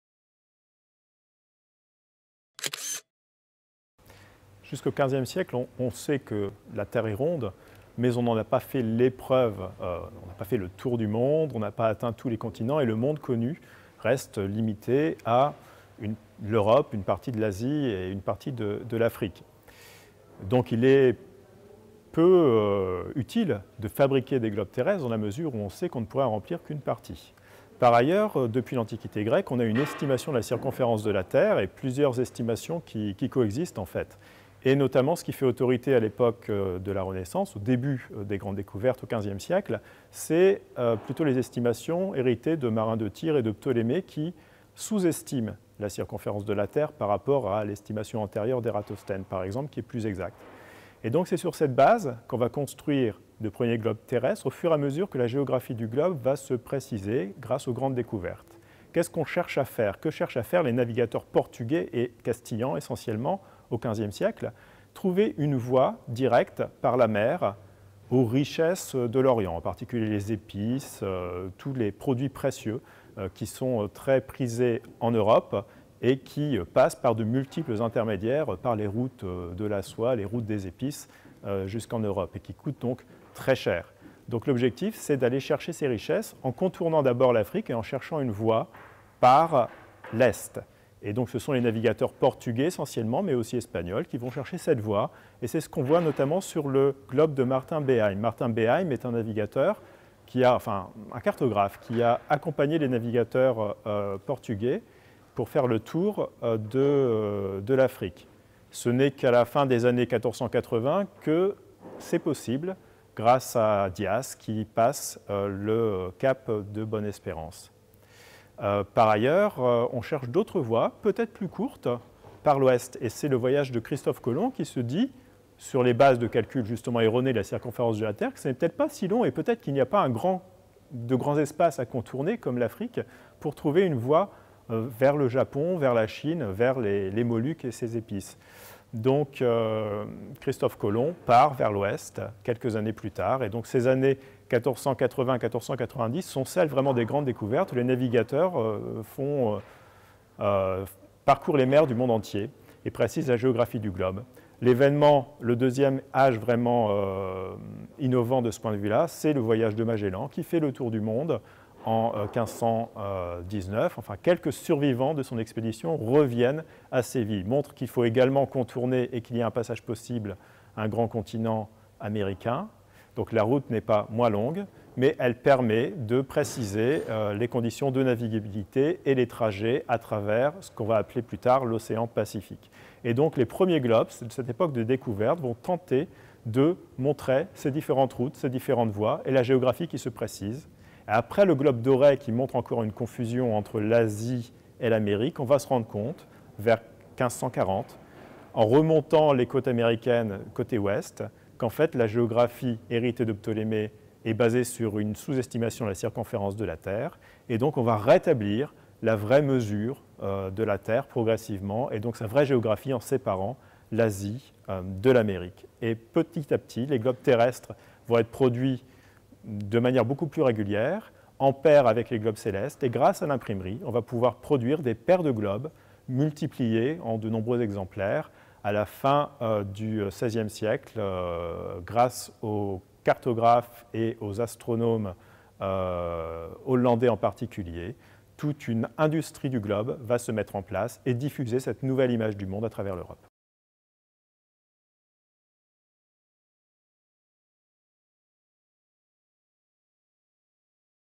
Le triomphe du globe
Jusqu’au XVe siècle, on sait que la terre est ronde, mais le monde connu reste limité à une partie de l’Asie et de l’Afrique. Il est donc peu utile de fabriquer des globes terrestres. À partir du XVe siècle, grâce aux navigateurs portugais et espagnols qui cherchent une voie d’accès aux richesses de l’Orient, la géographie de la terre se précise et on construit les premiers globes terrestres. C’est ce qu’on peut voir sur le globe de Martin Behaim, cartographe qui accompagne les navigateurs portugais pour faire le tour de l’Afrique. Dias passe le cap de Bonne Espérance à la fin des années 1480, puis dans les années 1490, Christophe Colomb trouve une autre voie par l’Ouest. Enfin, Magellan fait le tour du monde en 1519 et précise les conditions de navigabilité et les trajets à travers ce qu’on appellera l’océan pacifique. Les premiers globes vont tenter de montrer ces différentes routes et différentes voies. À la fin du XVIe siècle, grâce à l’imprimerie, toute une industrie du globe se met en place pour diffuser cette nouvelle image du monde.
Interview de François Nawrocki, conservateur en chef et directeur adjoint de la bibliothèque Sainte-Geneviève